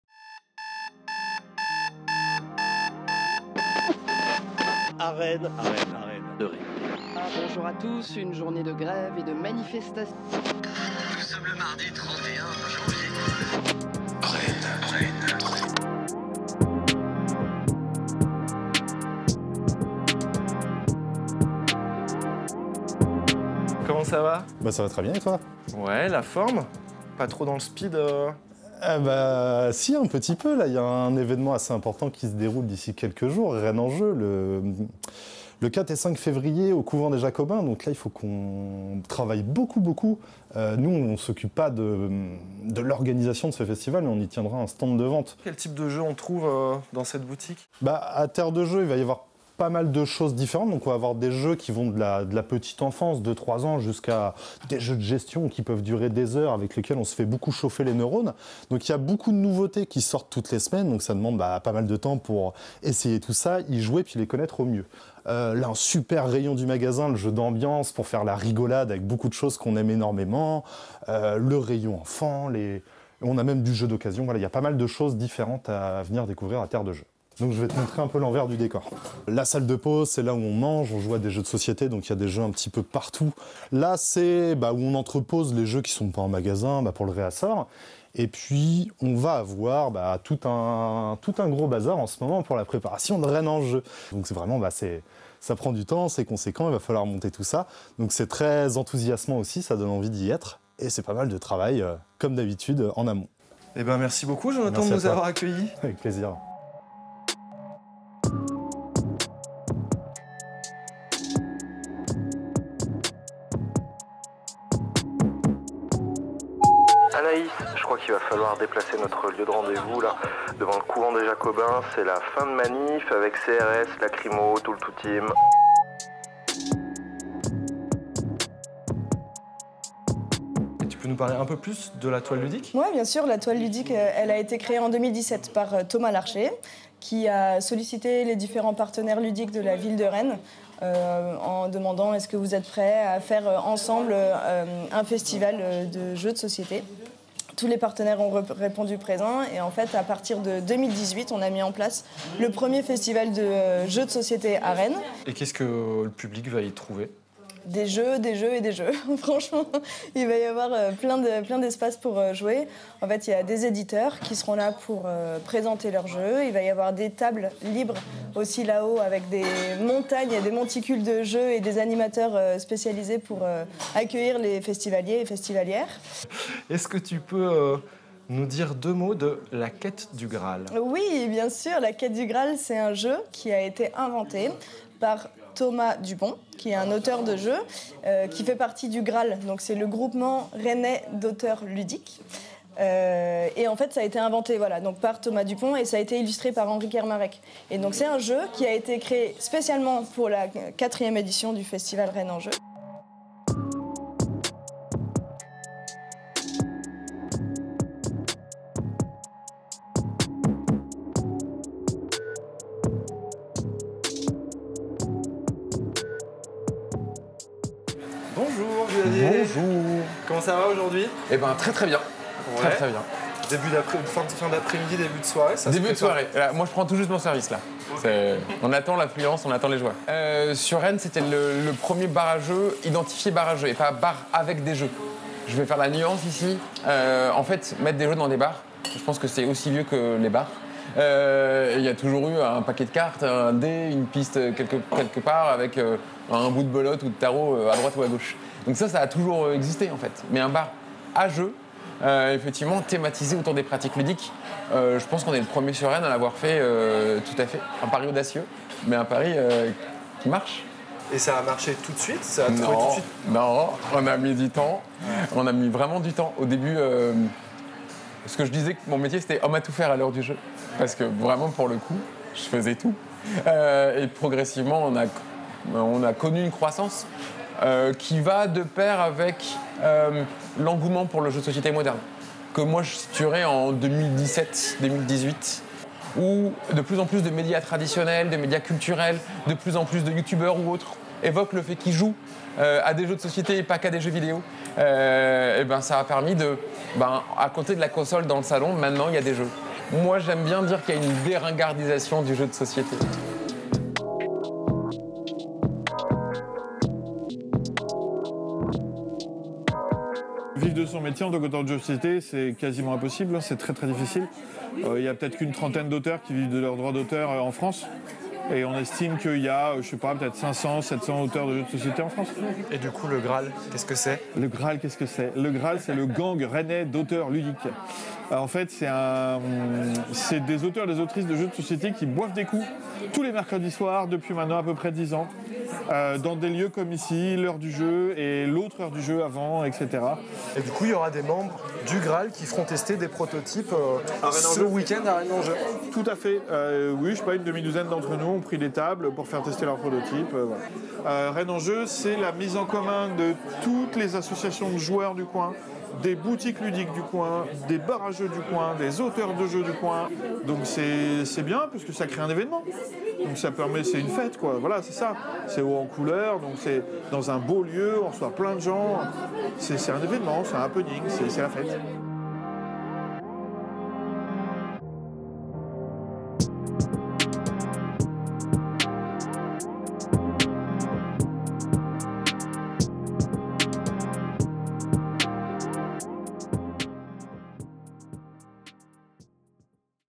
Un jour à Rennes / Webmag
Exercice de webmag de 6' réalisé dans le cadre de la formation Cadreur/monteur audiovisuel, à l'ESRA Bretagne.

Rédacteur, réalisateur, monteur, mixeur : Jean-Philippe Lecomte
Opérateur de prise de son et monteur : Léo Leroy.